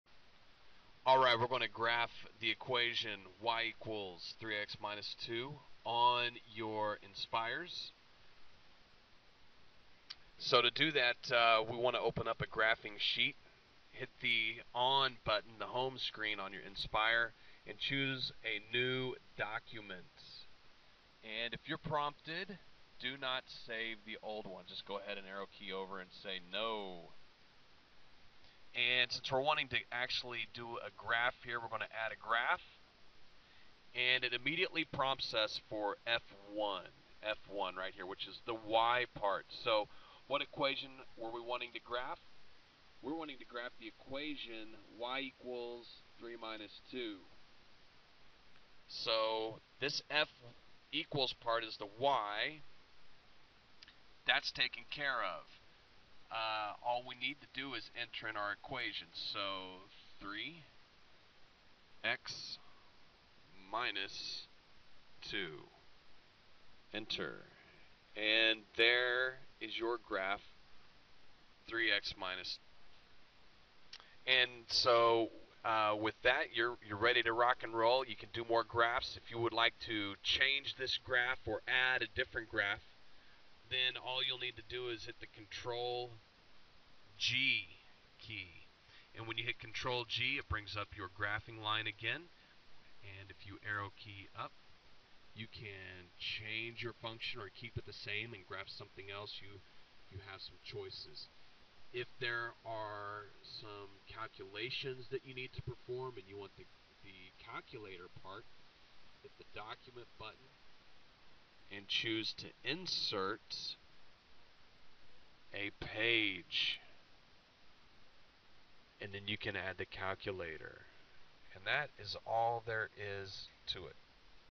Algebra 2 - Graphing Linear Equations TI NSpire.mp4
A quick lesson on graphing a linear equation with the TI Nspire